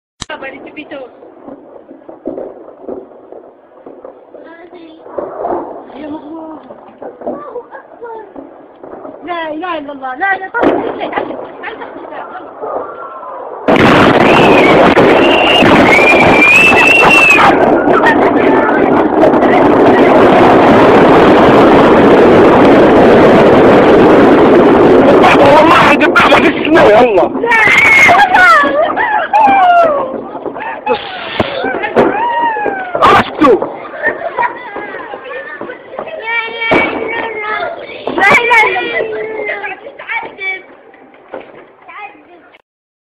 Ammo supply or gound cluster bomb? Brega, Libya - Raw Footage
Using a sledgehammer to crack a nut. Very scary for the women and children in Brega, Libya.